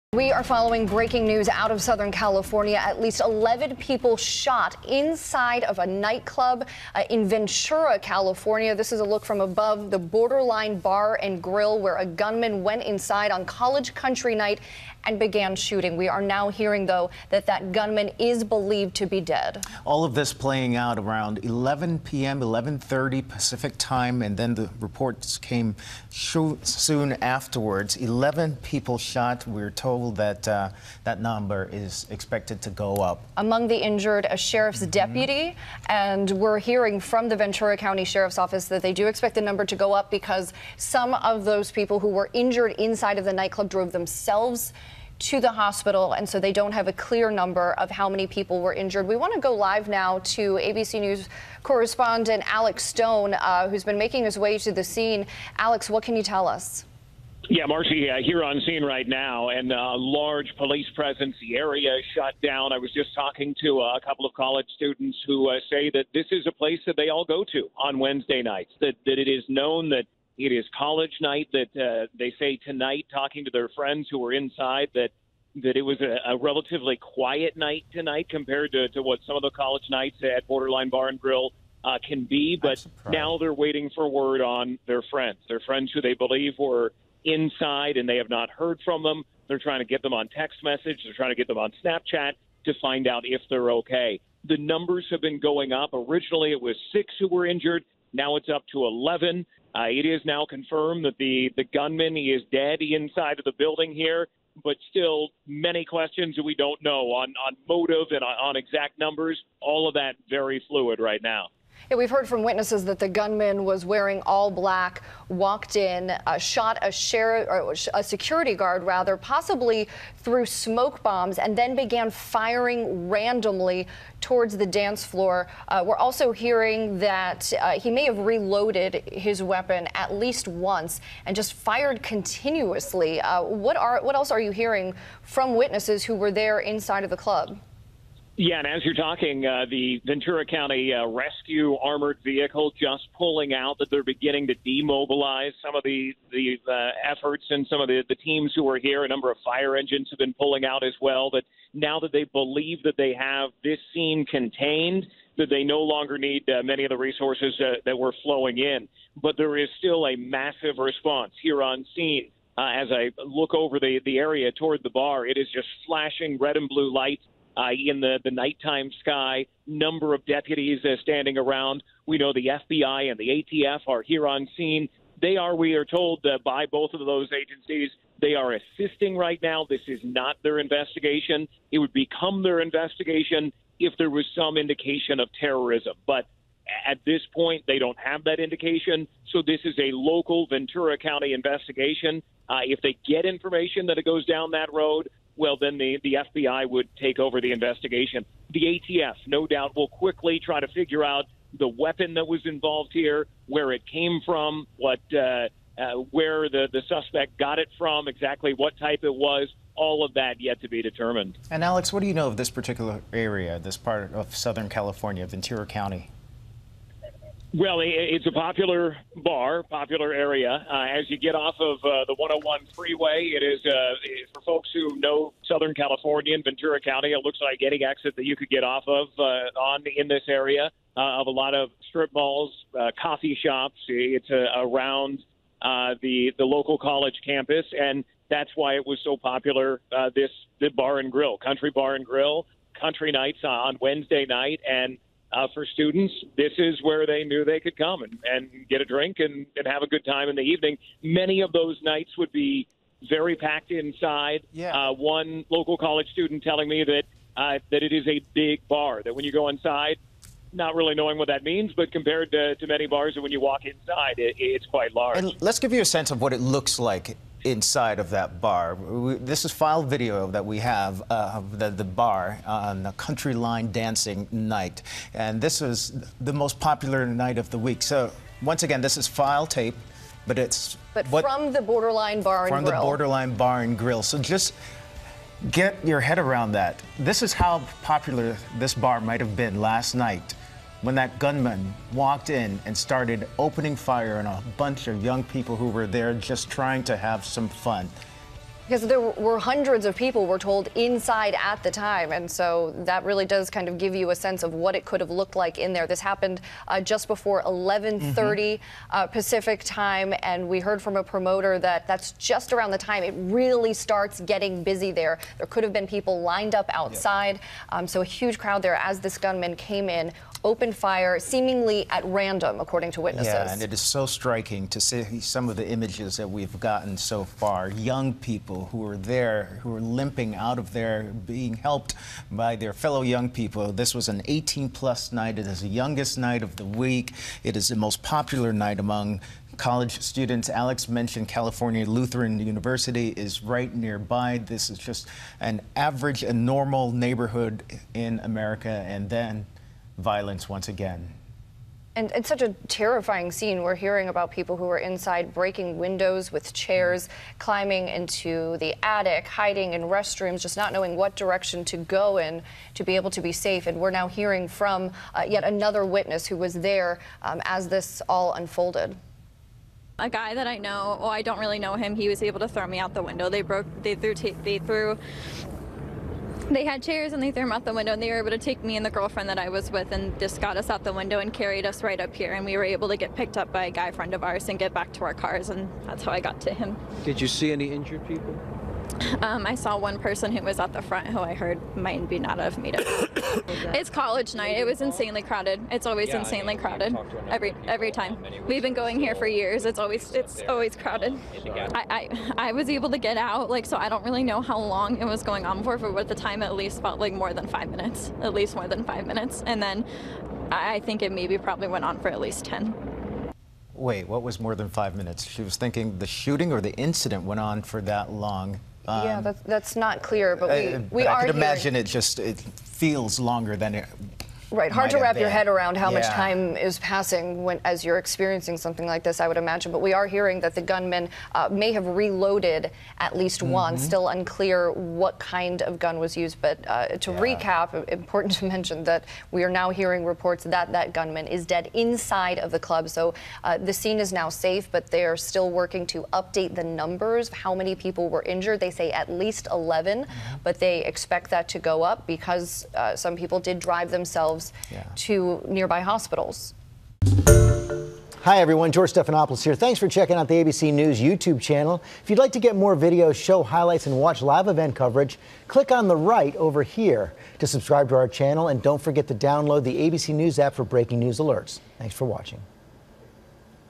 Mass Shooting At Borderline Bar And Grill In California
November 08, 2018 - The gunman is now believed to be dead after multiple people were shot during a "country night" for college students.